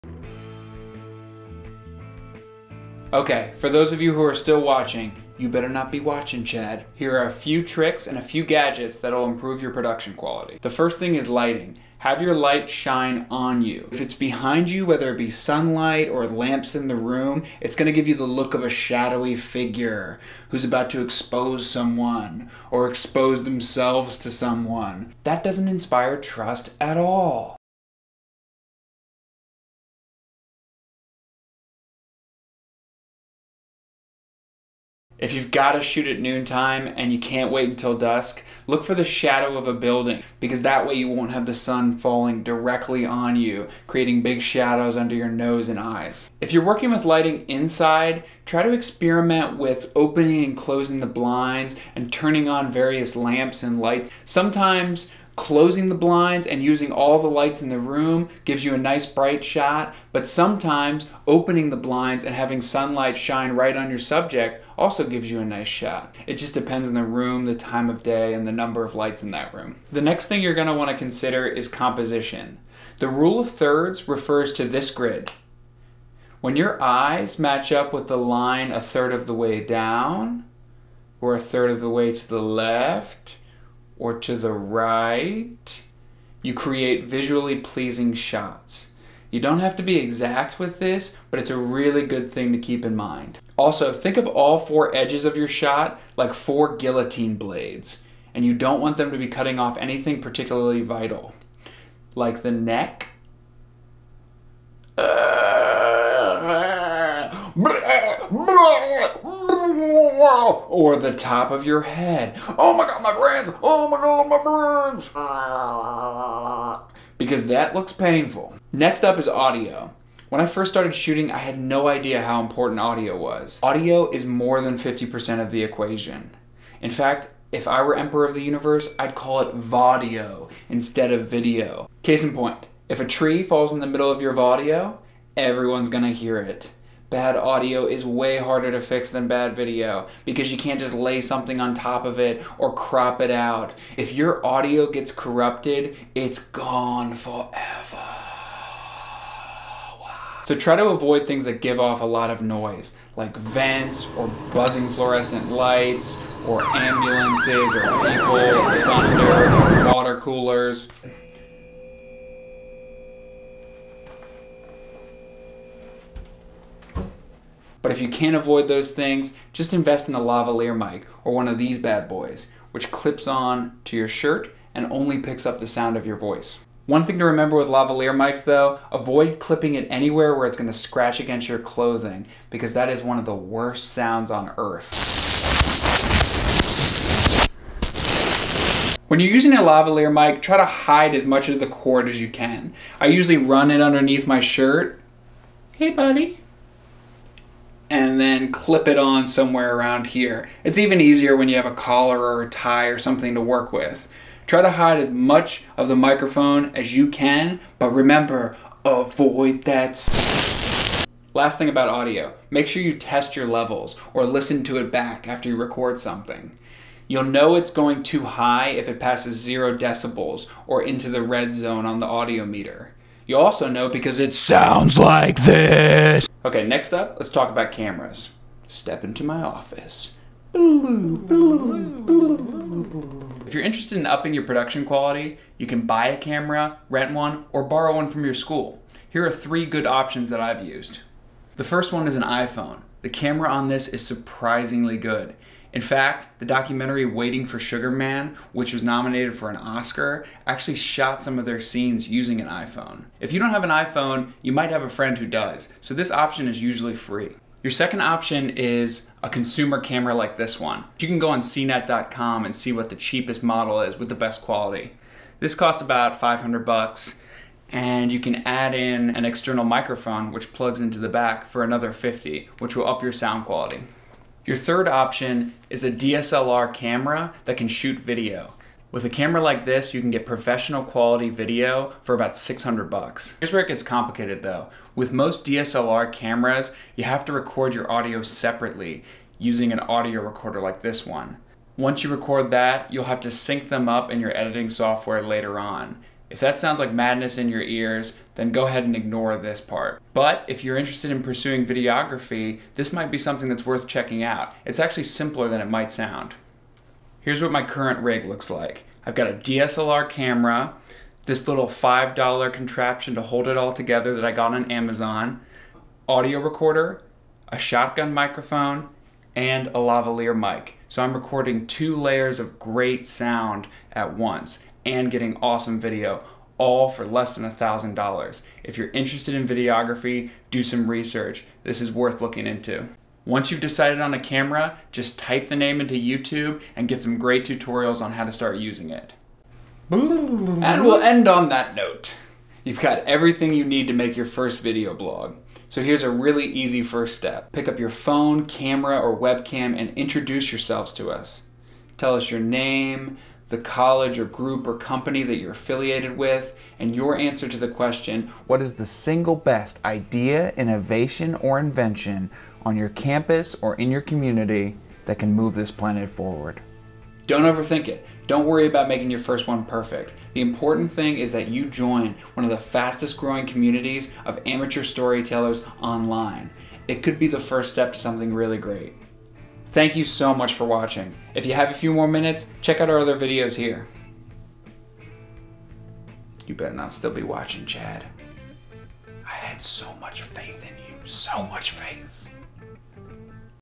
How to Make a Video Blog: Audio, Lighting, and Equipment
Once you know how to make a video blog, there are ways for you to make it really stand out. Composition and backgrounds can give you visually pleasing shots. Avoiding noise in your environment and watching your audio levels can keep people engaged. Investing in a little equiptment can help you get a “professional look.”

You are the star of the show, and what you say is the show itself, but a few smoke and mirrors fixes only amplify your voice. You don’t need to take on more than you’re comfortable with. A handheld phone can do the trick. This video outlines all the ways you can take your video further when you’re ready.